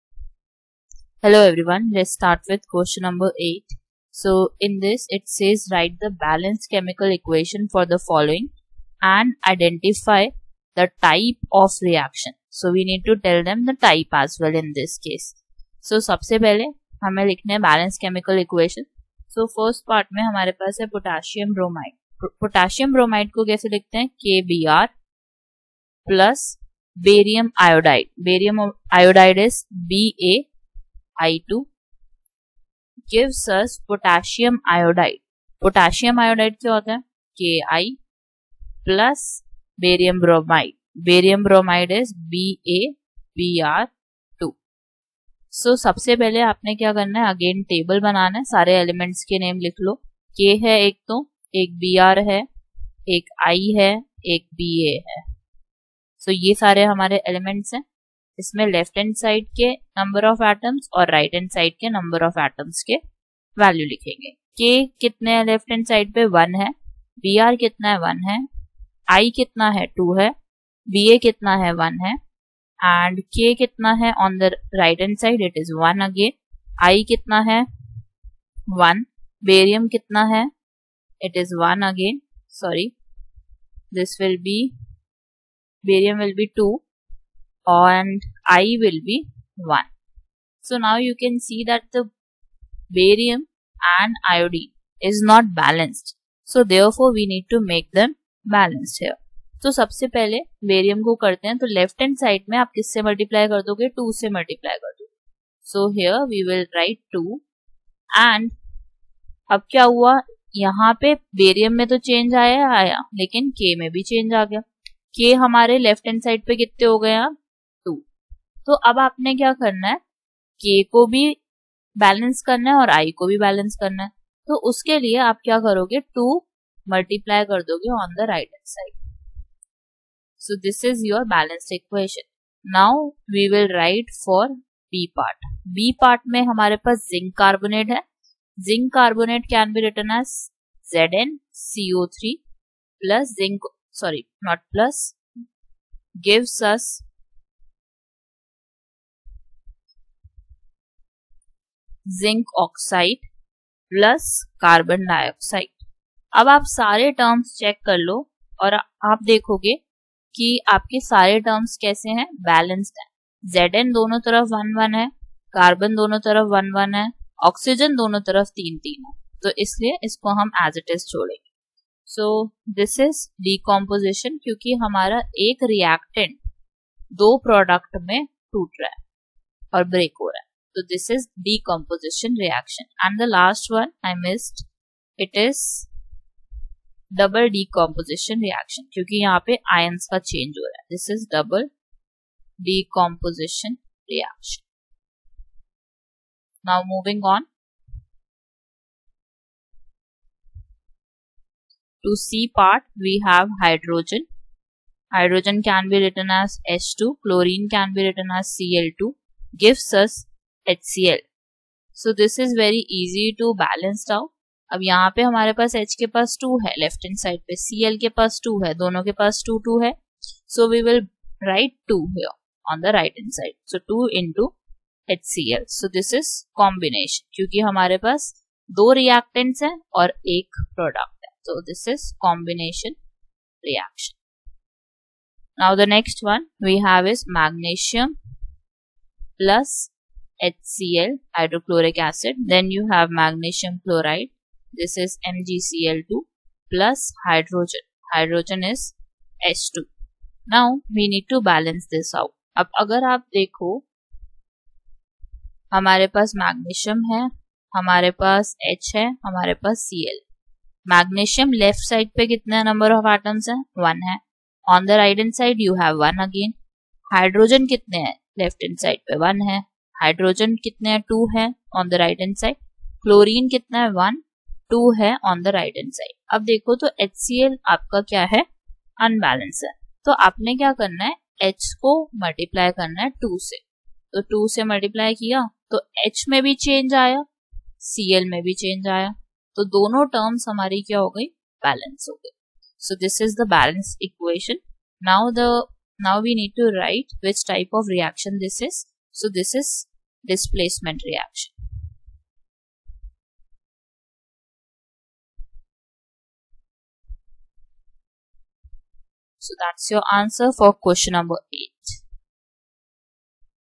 Q8 Class X Chapter Chemical Reactions and Equations Back exercise
8. Write the balanced chemical equation for the following and identify the type of reaction in each case.
(a) Potassium bromide (s) + Barium iodide (aq) → Potassium iodide (aq) + Barium bromide(s)
(b) Zinc carbonate (s) → Zinc oxide (s) + Carbon dioxide (g)
(d) Magnesium (s) + Hydrochloric acid (aq) → Magnesium chloride (aq) + Hydrogen (g)